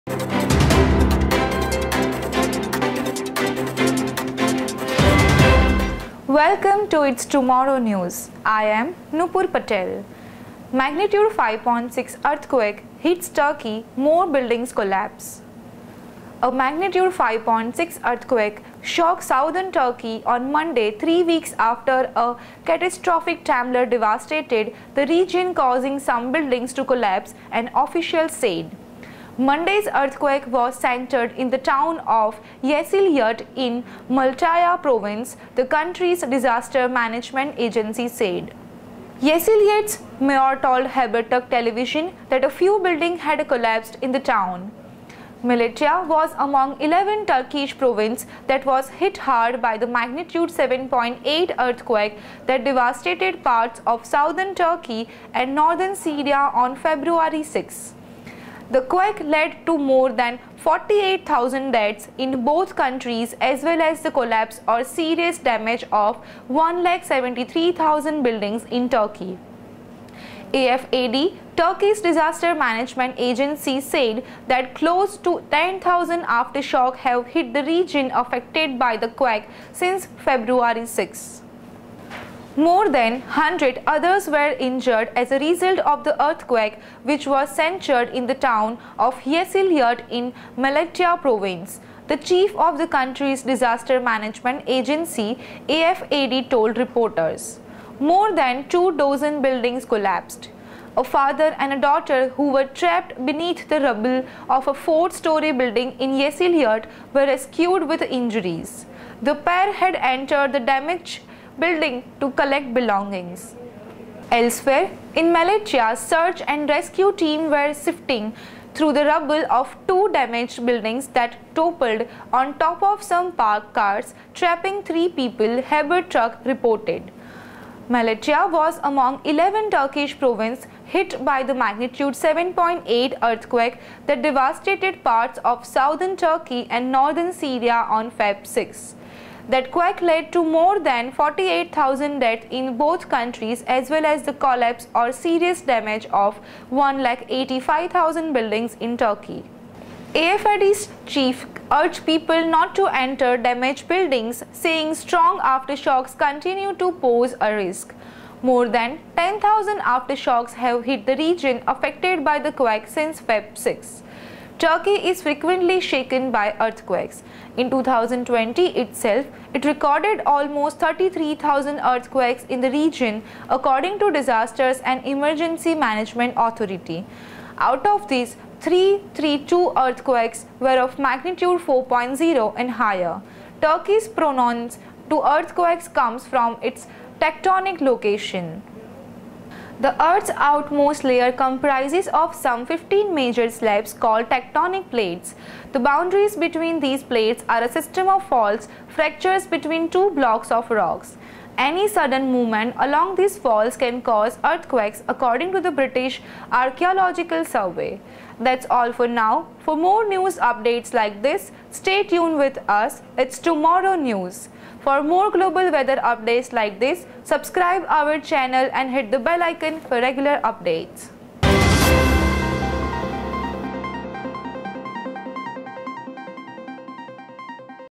Magnitude 5 6 earthquake hits Turkey; more buildings collapse | Turkey News | It's Tomorrow News
A magnitude 5.6 earthquake shook southern Turkey on Monday three weeks after a catastrophic temblor devastated the region causing some buildings to collapse, an official said. Monday’s earthquake was centered in the town of Yesilyurt in Malatya province, the country’s disaster management agency said.